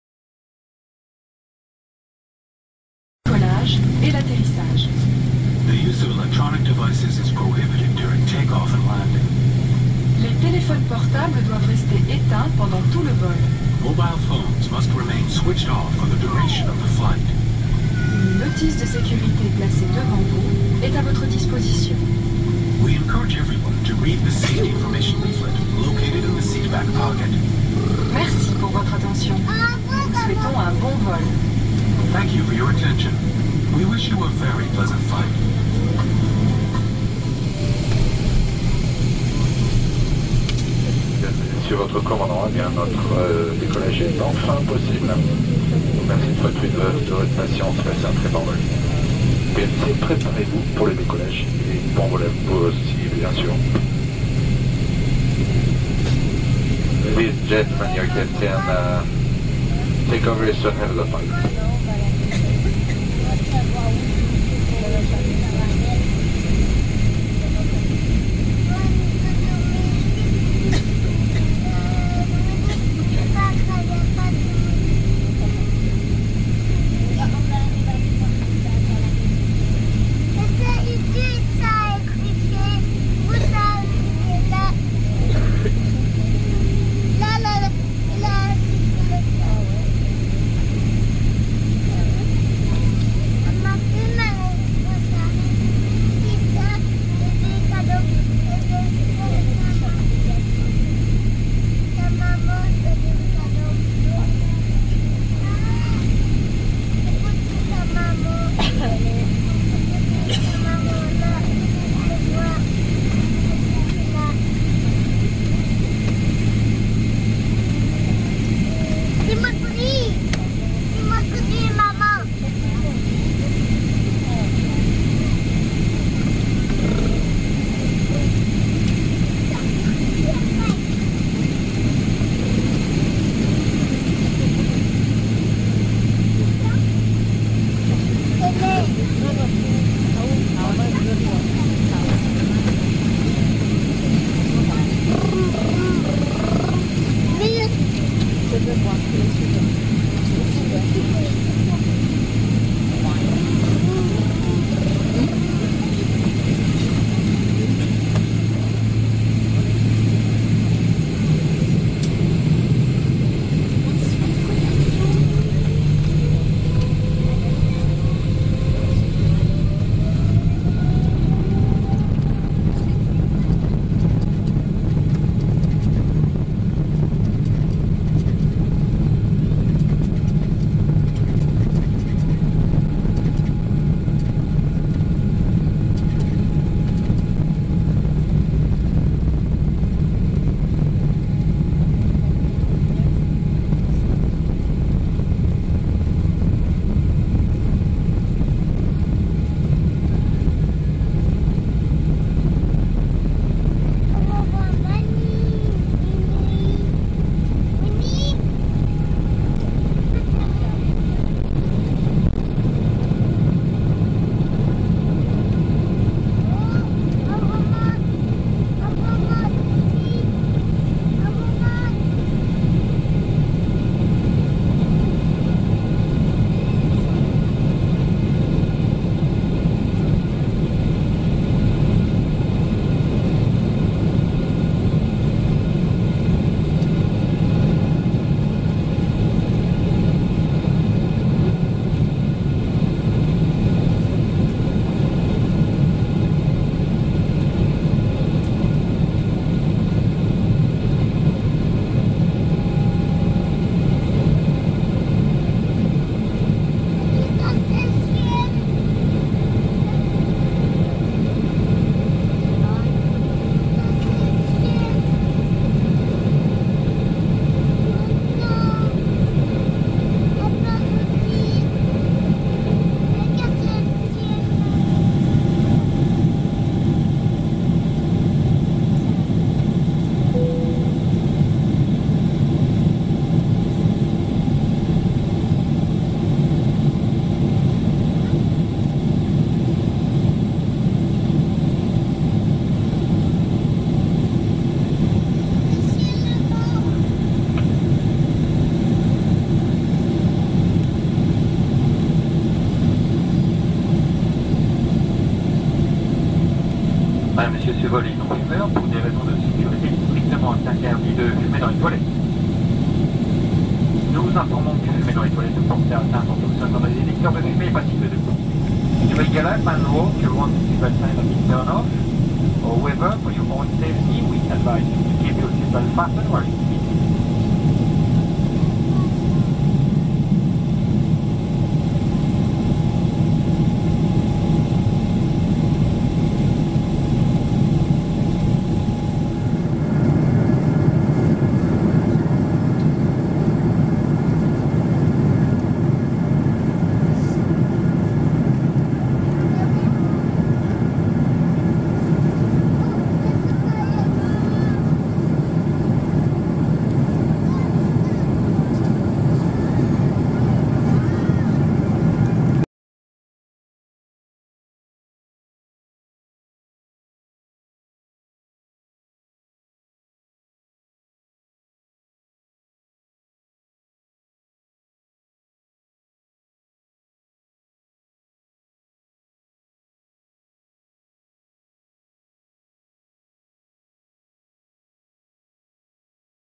Boeing 77W Take Off from Paris Orly
Aircraft : Air France Boeing 777-300ER
Airport : Paris Orly - France - LFPO/ORY
Cruise : FL340
Seat : 22L
Camera : Canon Powershot SX200IS
Date : August 25th 2009

Always a pleasure to fly Air France!